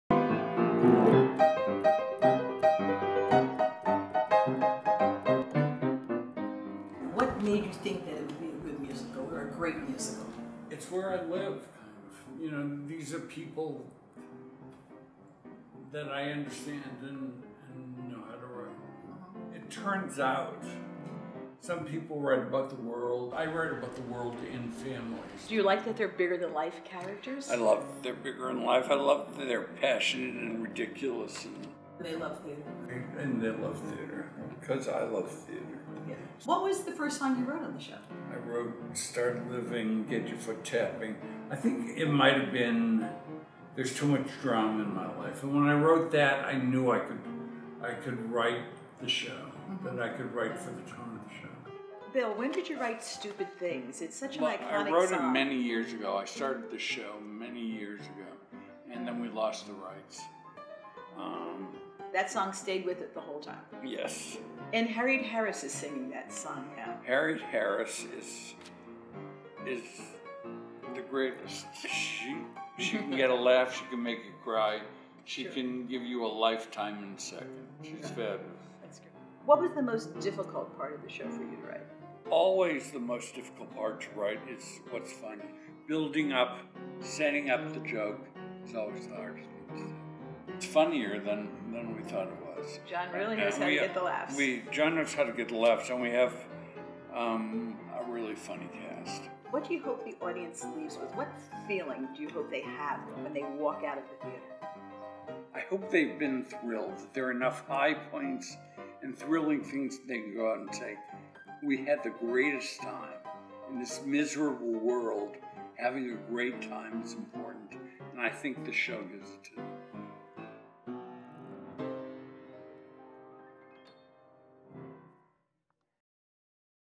William Finn on THE ROYAL FAMILY OF BROADWAY
Artistic Director Julianne Boyd interviews William Finn about his upcoming World Premiere musical, THE ROYAL FAMILY OF BROADWAY.